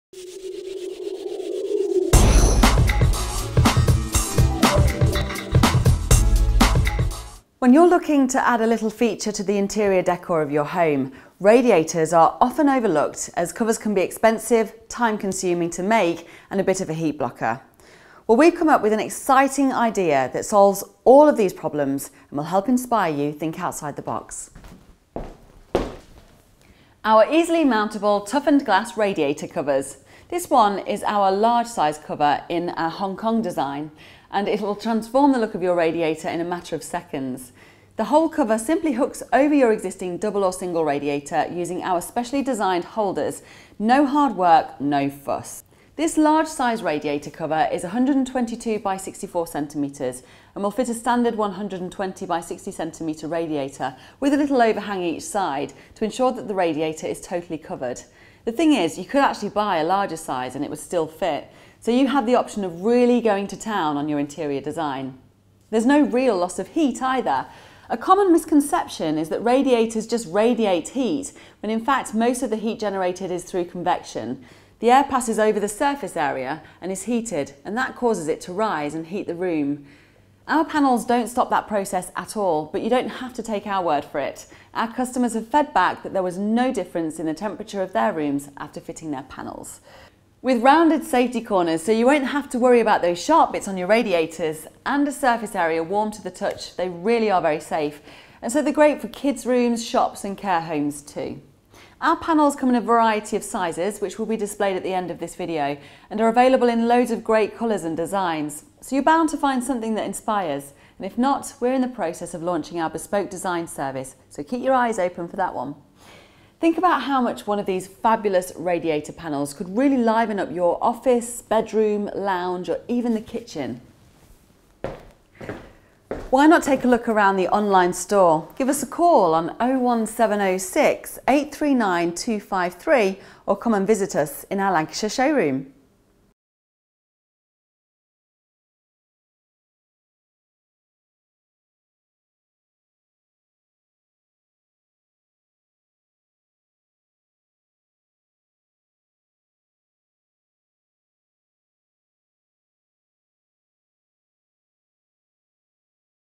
Radiator Cover - Hong Kong
When you’re looking to add a little feature to the interior décor of your home, radiators are often over looked as covers can be expensive, time consuming to make and a bit of a heat blocker.

Well, we’ve come up with an exciting idea that solves all of these problems … and will help inspire you to think outside the box – our easily mountable, toughened glass radiator covers.

With rounded safety corners and a surface area warm to the touch they are very safe – so they are great for kids’ rooms, shops and care homes too.

Our panels come in a variety of sizes, which will displayed at the end of this video,  and are available in loads of great colours and designs, so you’re bound to find something that inspires – if not, we’re in the process of launching our bespoke design service – so keep your eyes open for that.

Think how much one of these fabulous radiator panels could liven up your office … or bedroom … or lounge … even in the kitchen.

Why not take a look around the online store - give us a call, on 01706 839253 or come visit us in our Lancashire Showroom.